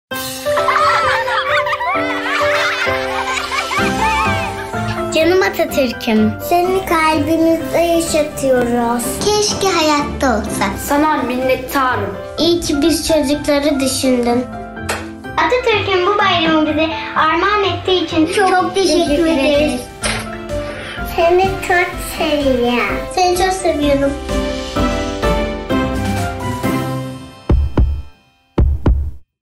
Söz Küçüğün; Atatürk’e ne söylemek isterdin?
Çocuklarımızın, 23 Nisan’ı onlara armağan eden Atamıza söyleyecekleri var.
23 Nisan Ulusal Egemenlik ve Çocuk Bayramımızı ve Meclisimizin kuruluşunun 100. yılını gönülden kutluyor, sözü miniklere bırakıyoruz.